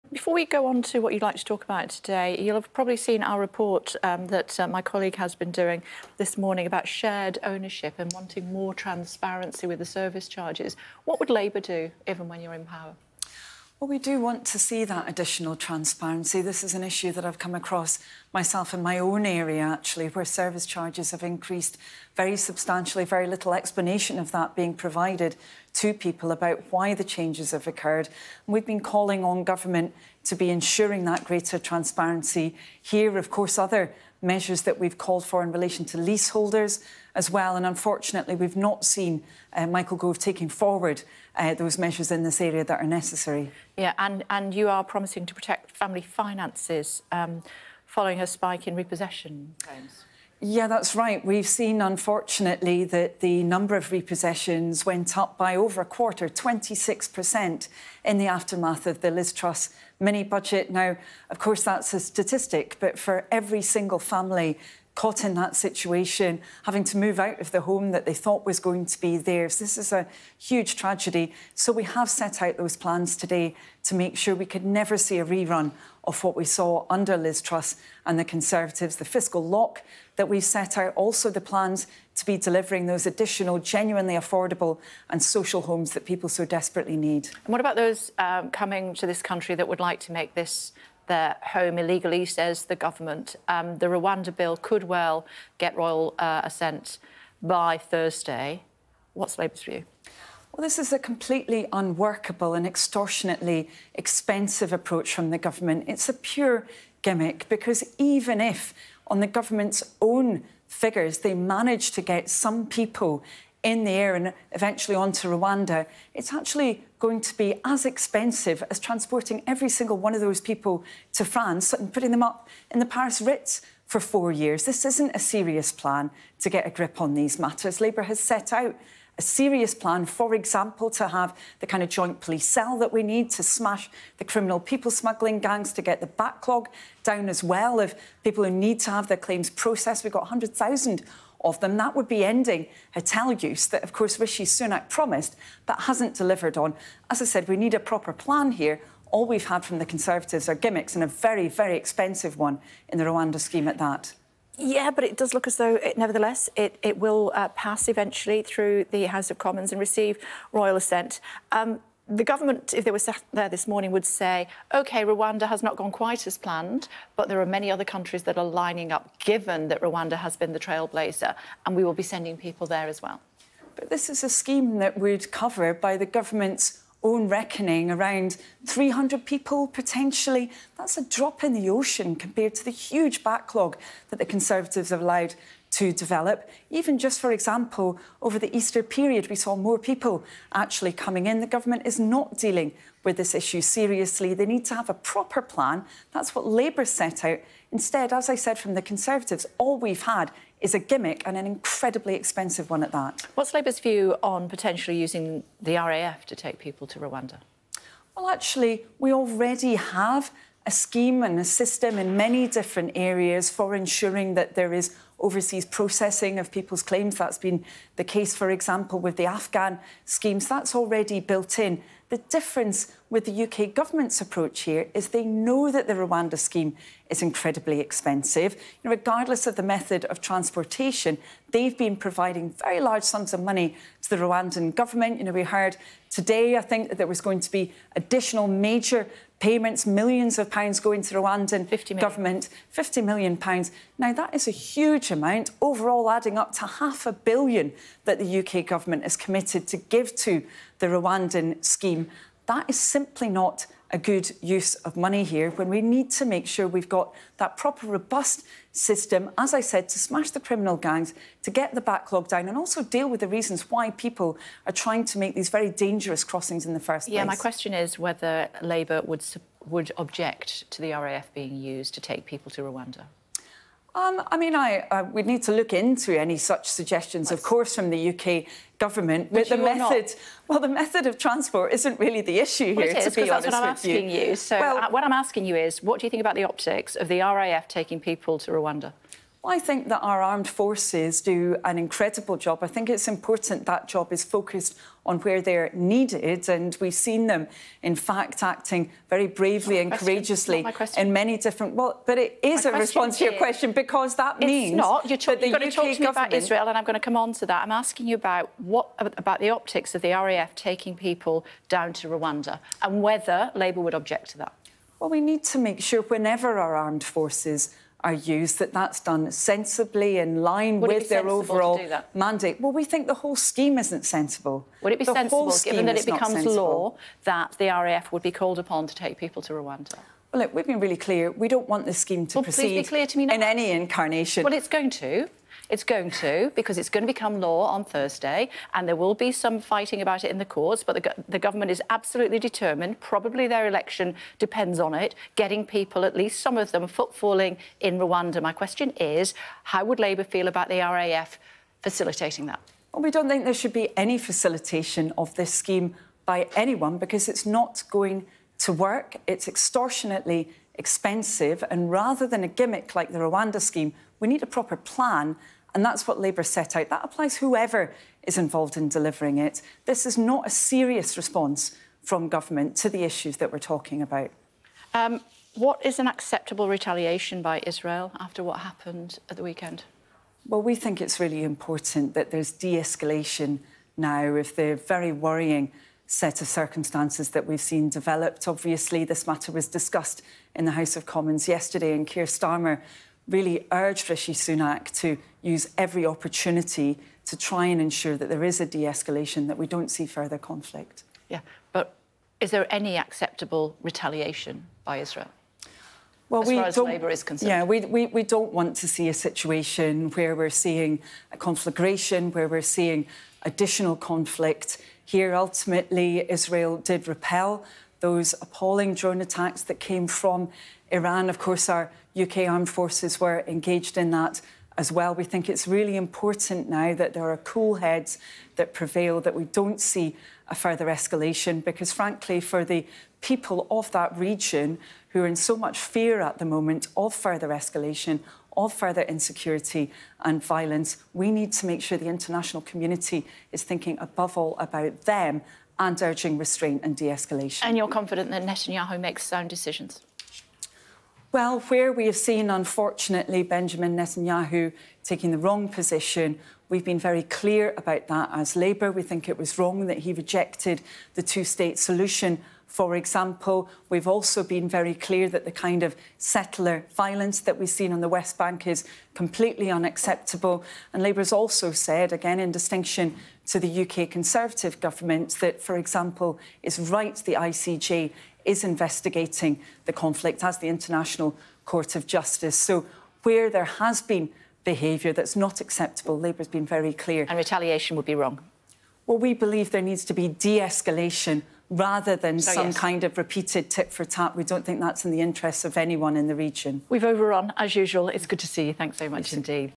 'Greater transparency' needed in shared ownership costs, says Labour party chair
Anneliese Dodds, chair of the Labour Party, has urged the government to push for "greater transparency" in costs under shared ownership.

Asked about our report that service charges are soaring, Ms Dodds says: "We do want to see that transparency."